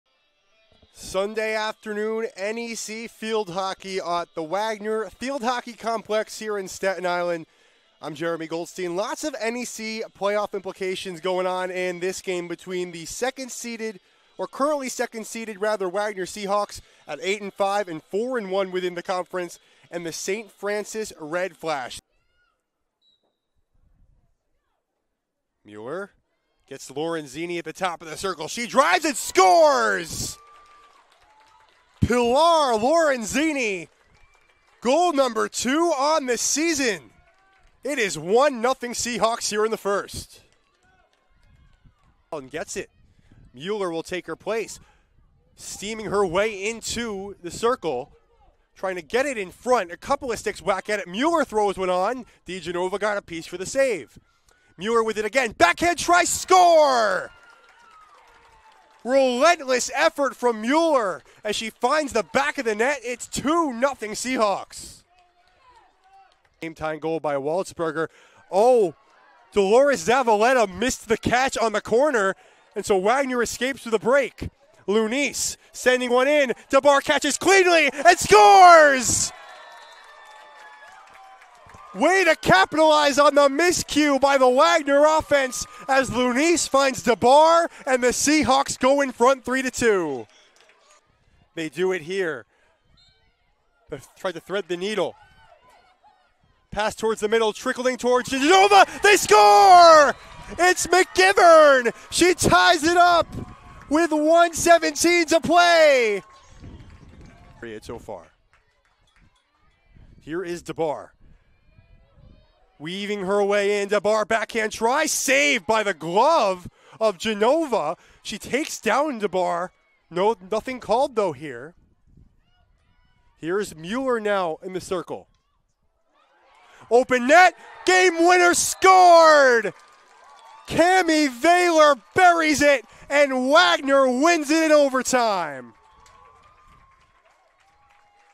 Highlights: Wagner Field Hockey vs. Saint Francis U
Check out the top plays from the Wagner's huge win over on Saint Francis U on Grymes Hill!